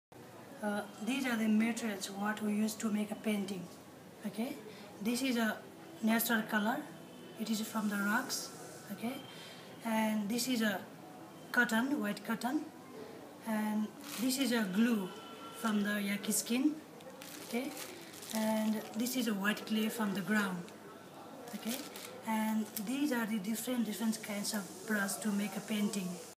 What materials are used in thangka paintings?
Suresh Lama of the Tushita Thankga Artists' Co-operative lists the materials used in thankga (Tibetan silk scroll) painting.